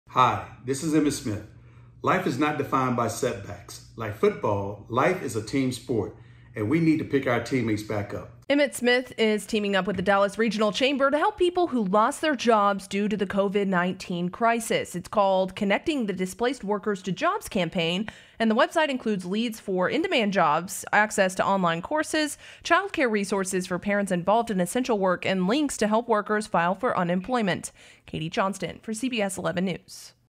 Emmitt Smith Joins Forces With The Dallas Chamber To Help North Texas Workers Find Jobs
The Dallas Regional Chamber has started a new initiative to connect workers displaced by the COVID-19 crisis to thousands of available jobs and they've brought on former Dallas Cowboy Emmitt Smith to help. Katie Johnston reports.